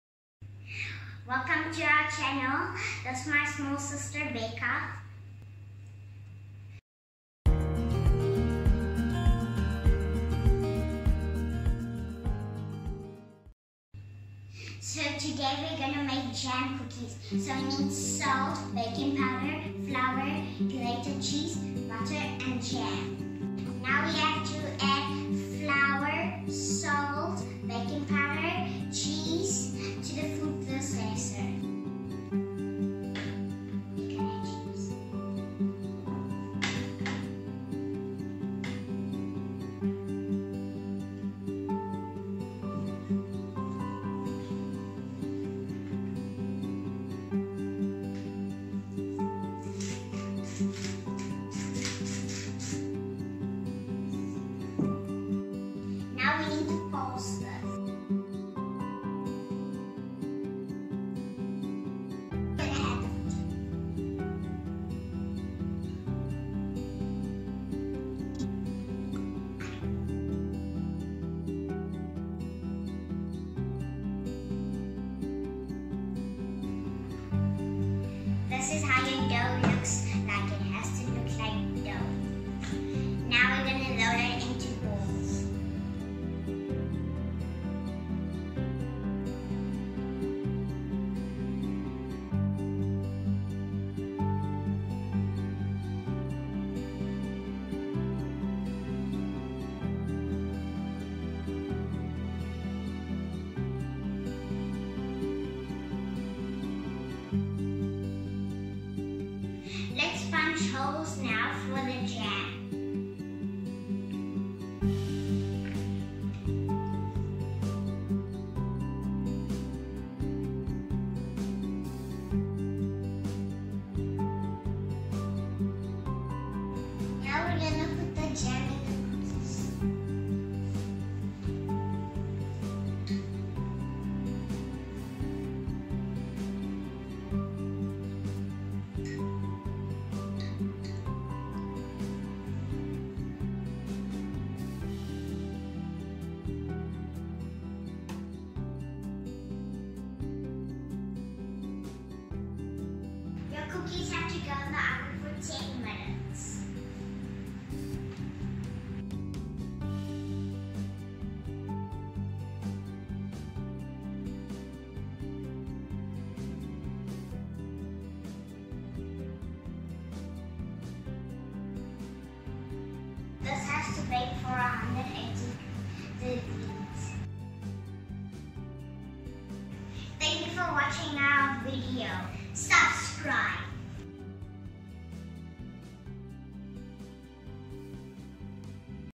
Thumbprint Cookies
Haniya was busy and I wanted to bake so I asked my little sister to bake with me.
Here's How:-

250ml Flour
1/4 teaspoon salt
1/4 teaspoon baking powder
350ml/120g grated cheese
125ml/120g soft butter

Method

1. Pre-heat the oven at 180 degrees Celcius.

2. Put the flour, salt, baking powder, butter and cheese in a food processor

3. Pulse until it forms a ball of dough

4. Roll small balls and place on a greased baking pan

5. Use the back  of a wooden spoon or any small round object to punch a hole in the middle of each cookie

6. Put a small amount of any flavor jam in the holes

7. Bake for 10-12 minutes